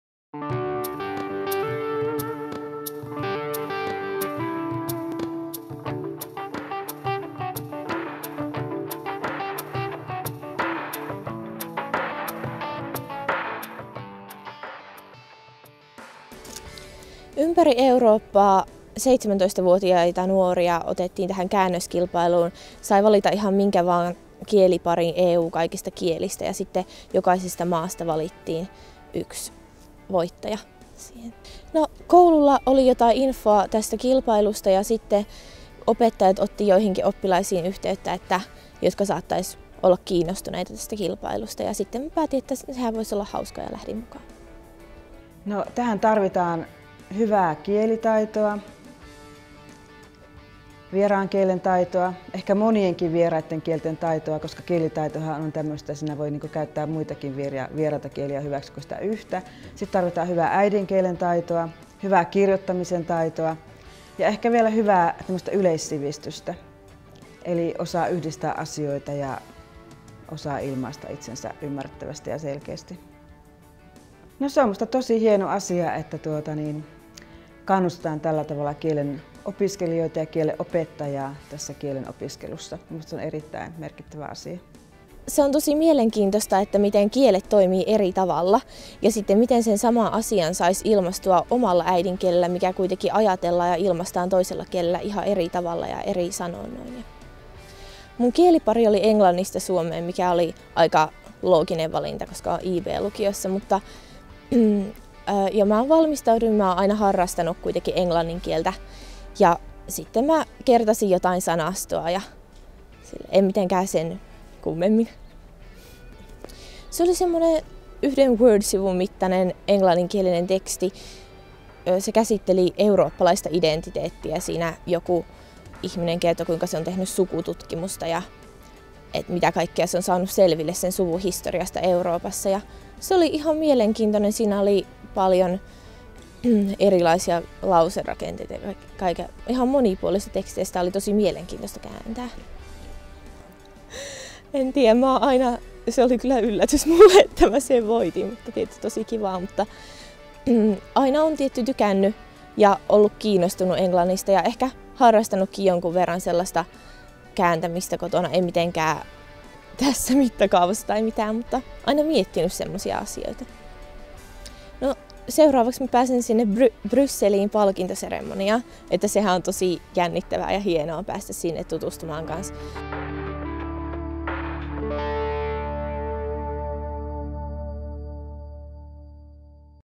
Käännöskilpailun voitto Jyväskylään
Euroopan komission vuosittaisen Juvenes Translatores -käännöskilpailun voitti tänä vuonna Hanna Lehmonen Jyväskylän Lyseon lukiosta. Helmikuussa 2015 kuvatulla videolla hän ja opettaja Ulla Aarnio kertovat kilpailusta.